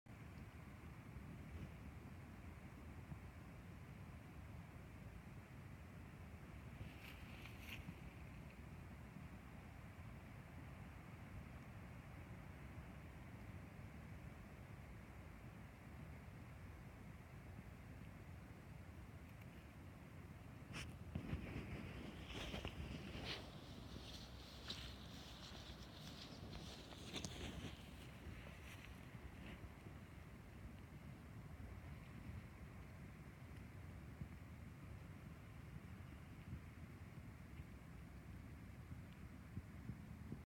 Bruyère alpine 'Erica carnea Aurea' Le froid la rend si jolie
Feuillage fin persistant
Bien touffu consistant
Couvre sol tapissant
Tout l'hiver fleurissant